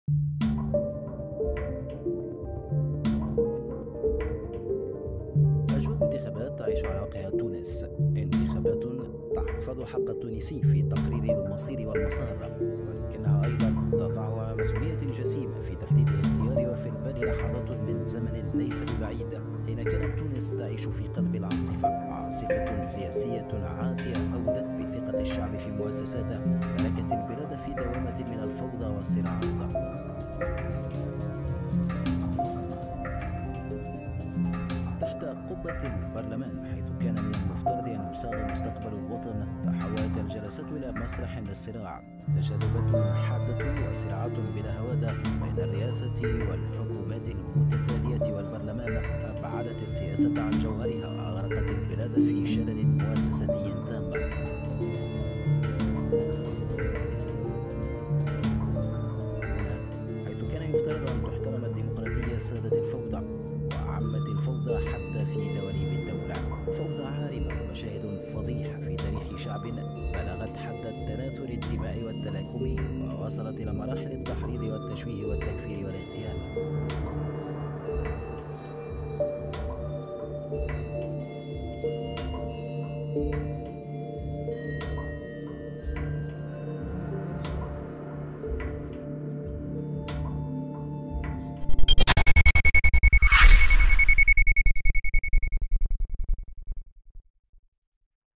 نواة في دقيقة: تطبيل اعلامي رديء بأموال دافعي الضرائب
في الوقت الذي تتسارع فيه أخبار قمع الحريات ولجم الافواه والتنكيل بالمعارضين، أبهرتنا أم الإذاعات الممولة من دافعي الضرائب بتقرير مصور غاية في الرداءة والتطبيل. تقرير من المفترض تدريسه في معاهد الصحافة كمرجع للانحدار الإعلامي منقطع النظير.